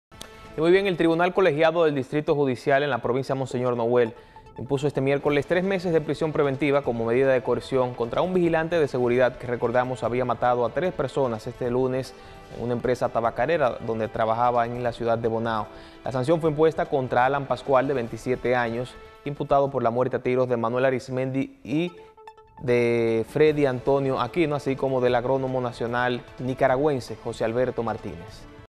DICTAN TRES MESES DE PRISIÓN PREVENTIVA A VIGILANTE QUE MATÓ 3 COMPAÑEROS EN BONAO.
El Tribunal Colegiado del Distrito Judicial de la provincia Monseñor Nouel impuso este miércoles tres meses de prisión preventiva, como medida de coerción, contra un vigilante de seguridad que mató a tres personas el pasado lunes en una empresa tabaquera de la ciudad de Bonao. La sanción fue impuesta contra Alan Pascual, de 27 años, imputado por la muerte a tiros de Manuel Arismendy García Jiménez, de 58 años; Freddy Antonio Aquino, de 63, y el agrónomo de nacionalidad nicaragüense José Alberto Martínez, de 42.